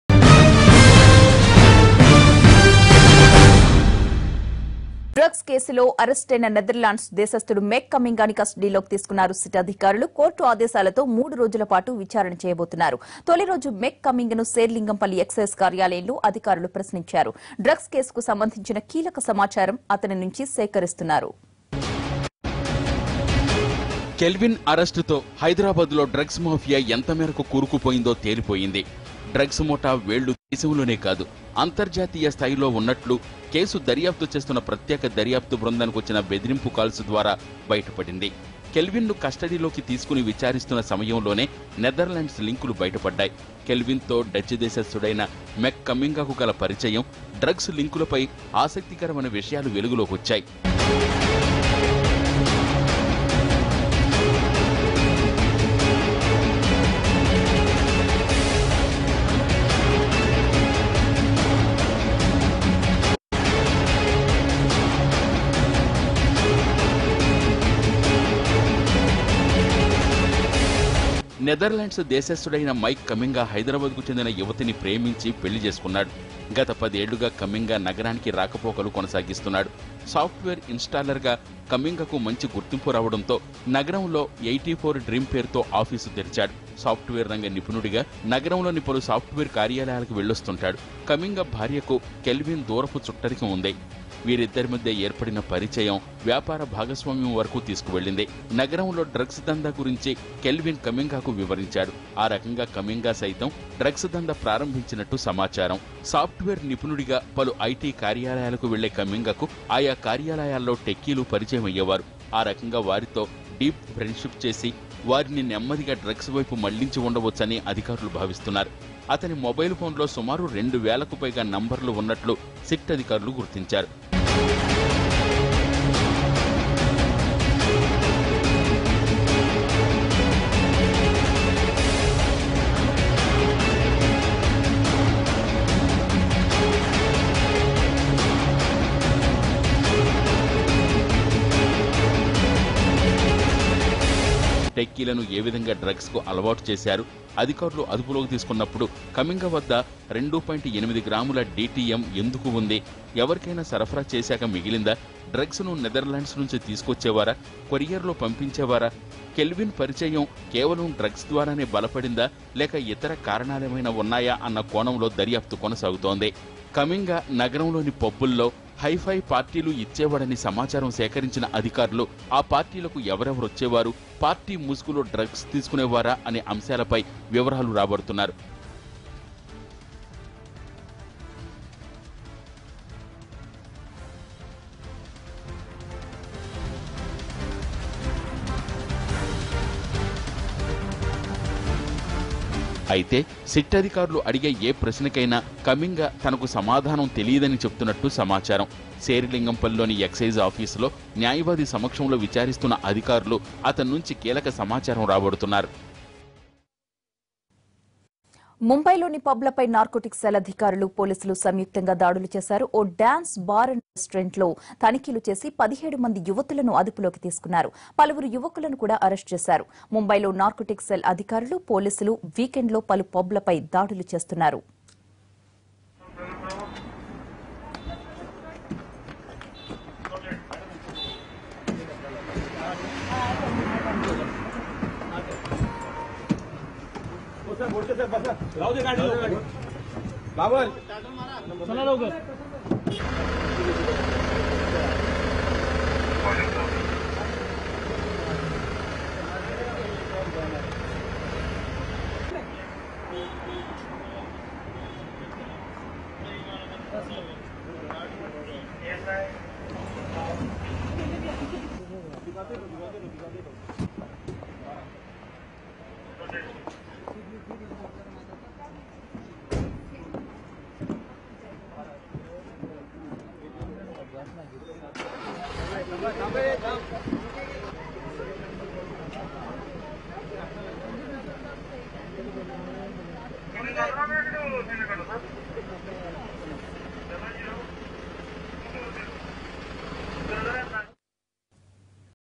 SIT Interrogates Narcotics Dealer Mike Kamminga || Hyderabad Narcotic Case || NTV
SIT Interrogates Narcotics Dealer Mike Kamminga || Hyderabad Narcotics Case || NTV
For more latest updates on news :